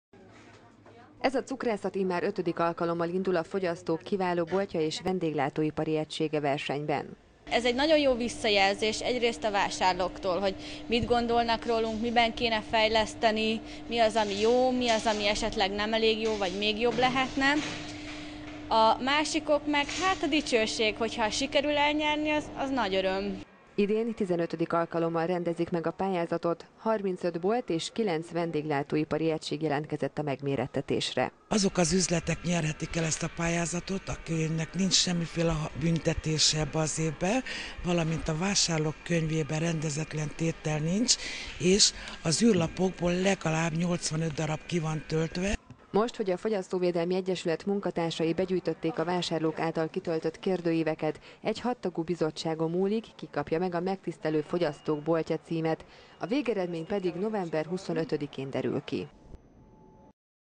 Melyik bolt lesz kíváló?
Célegyenesbe fordult a fogyasztók kiváló boltja és vendéglátó ipari egysége címért vívott verseny. A Székesfehérvári Fogyasztóvédelmi Egyesület munkatársai elkezdték begyűjteni a fogyasztók által kitöltött űrlapokat, amelyeken értékelhették a boltokat és a vendéglátó ipari egységeket, fogyasztói szemmel. Hamarosan tehát kiderül, ki kapja meg idén a megtisztelő Fogyasztók Boltja címet.